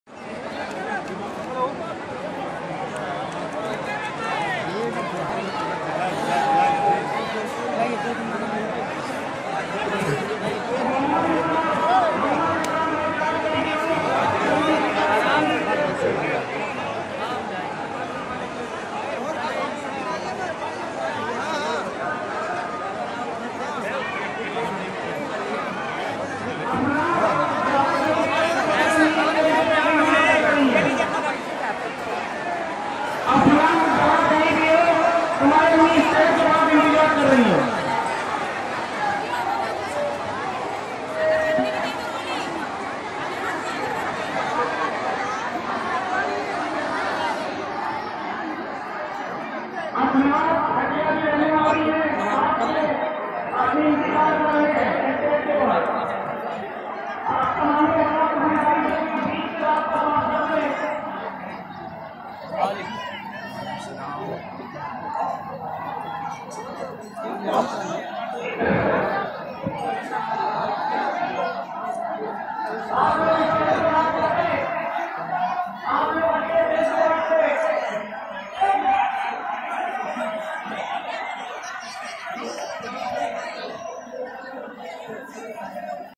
Dargah Shareef kydganj Allahabad Urs Hazrat Shah Muhibbullah Dada Miya RH
Our city Allahabad is so blessed to have Shah Mohibullah Allahabadi the descendent of the noted Sufi Saint Baba Fareed stationed here for his life and from here only he preached and operated Indo-Islamic movement.